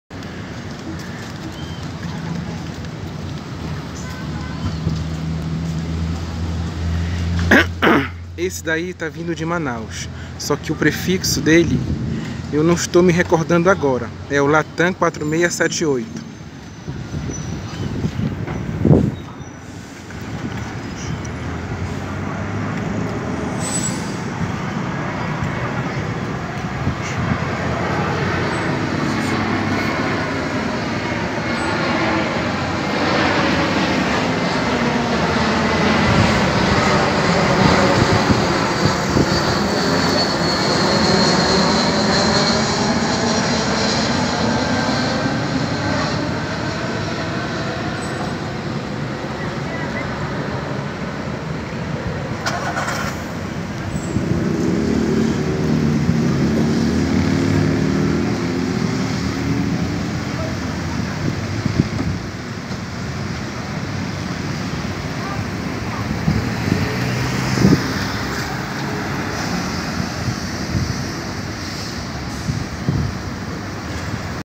[SBFZ Spotting]Airbus A321 PT-MXG na final antes de pousar em Fortaleza vindo de Manaus(05/03/2020)
Número de serie:4358
Tipo:A321-231
Data do primeiro voo:21/06/2010
Prefixo de testes:D-AVZK
Motores:s 2 x IAE V2533-A5
Data de entrega:06/07/2010
Fonte: airfleets.net/